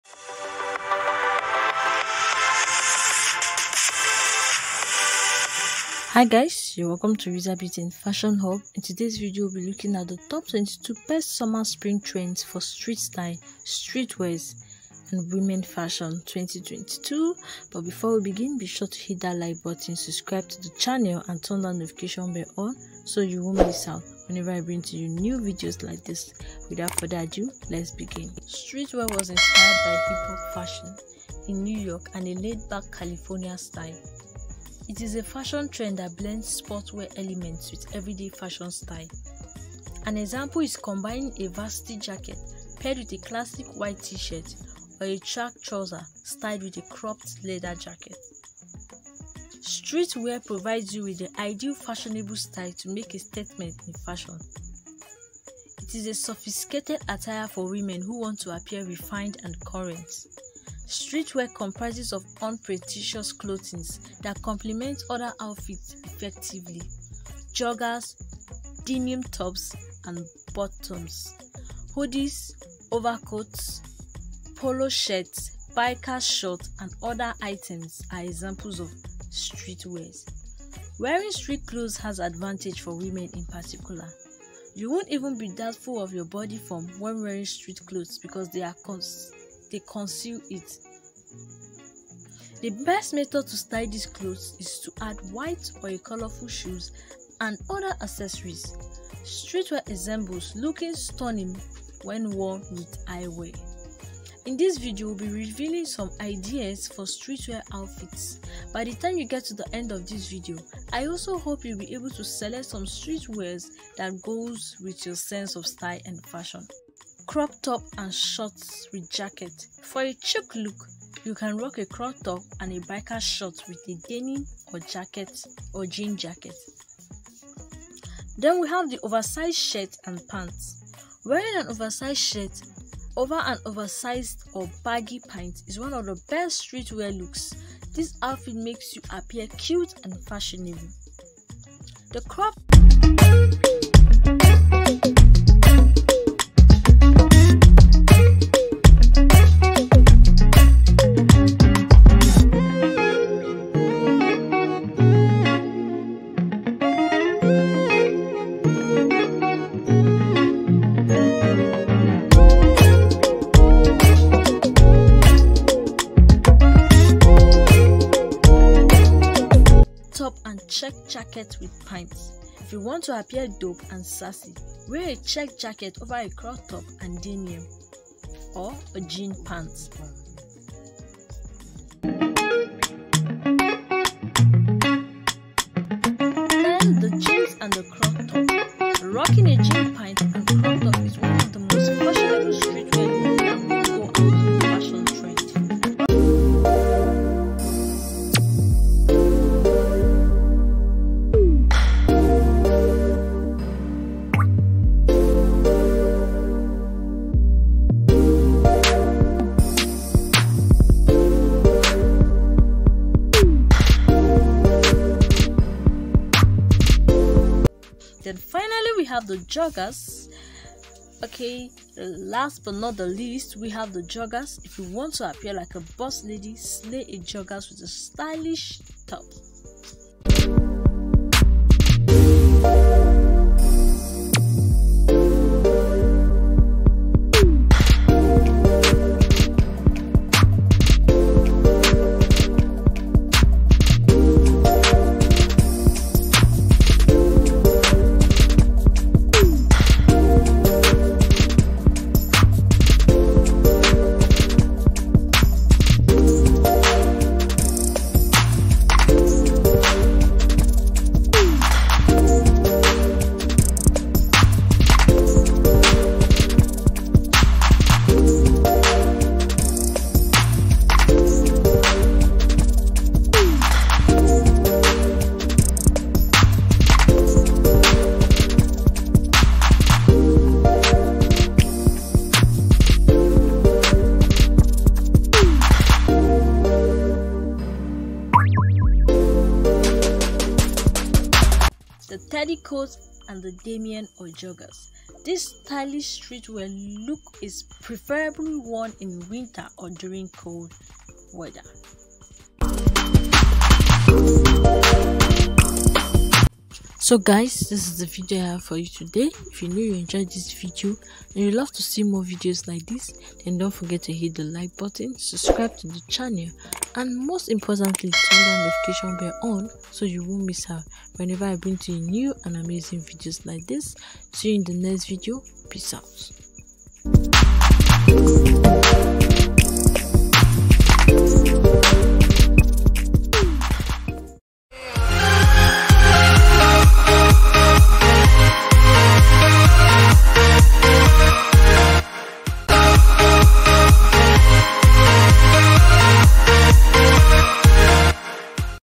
TOP 2022 BEST SUMMER/ SPRING TRENDS FOR STREET STYLE, STREET WEAR, WOMEN'S FASHION
In today's video we will showcasing TOP 2022 BEST SUMMER/ SPRING TRENDS FOR STREET STYLE, STREET WEAR, WOMEN'S FASHION

In this video you will discover stunning street wear styles
that will make you look Sassy and Elegant